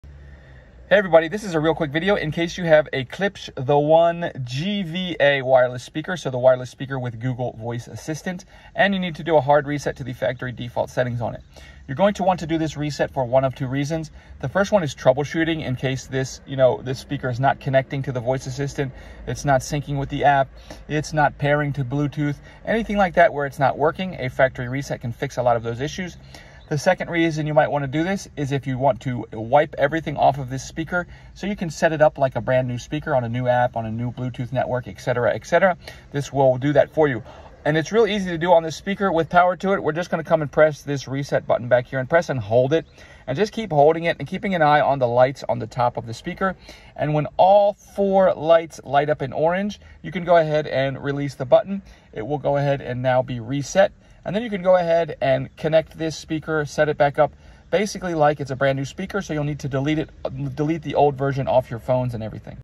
Hard Reset Klipsch The One Google Voice Assistant Speaker
How to troubleshoot or setup (pair) starting with a force hard factory reset Klipsch The One GVA Google Voice Assistant Bluetooth Smart Speaker to factory default settings if it is not working or not connecting/pairing or if it is frozen or not responding, the factory reset is a good troubleshooting step. You can also restore the factory settings to setup your device as new. Found in the Klipsch The One Google Voice Assistant Smart Speaker user manual.
Time for a new Bluetooth speaker?